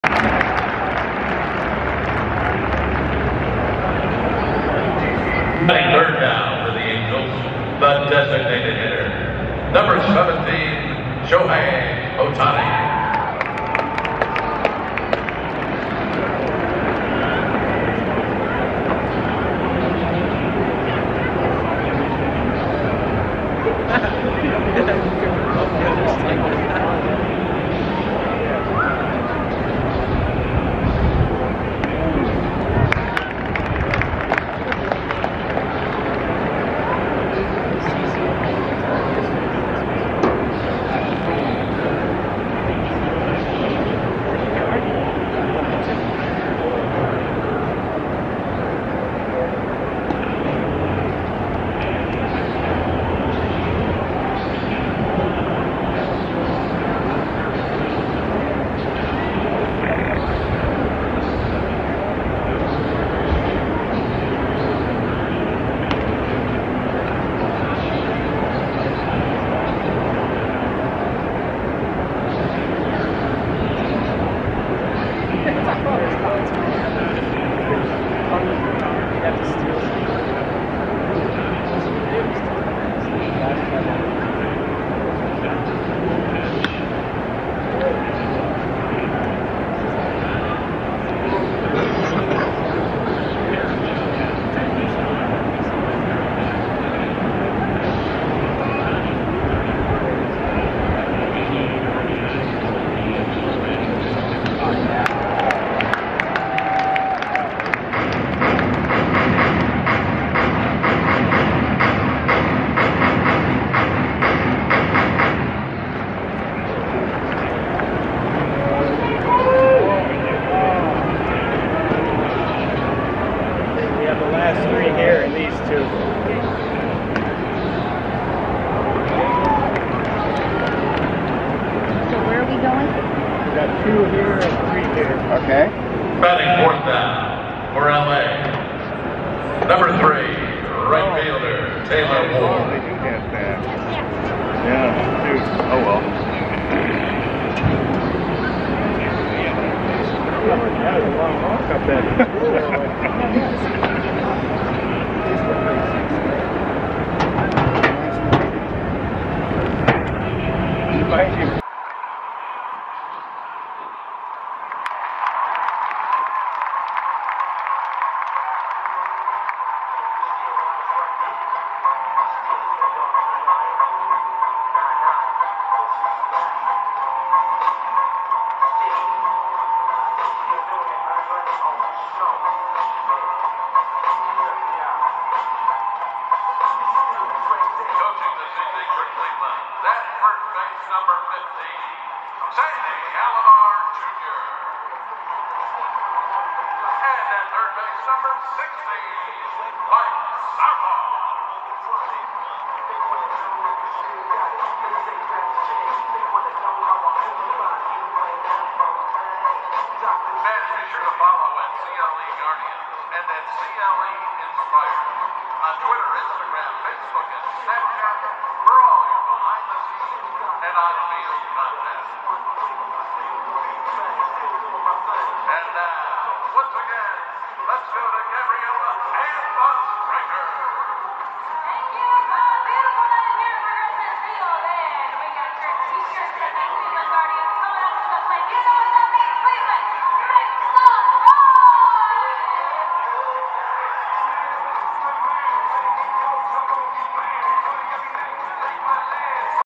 速報！ガーディアンズ第2戦！ 大谷翔平 第1打席【9.13現地映像】エンゼルス0-0ガーディアンズ3番DH大谷翔平 1回表2死ランナーなし場面#大谷翔平#第1打席#現地映像＃ガーディアンズ
エンゼルス0-0ガーディアンズ
3番DH大谷翔平　1回表2死ランナーなし場面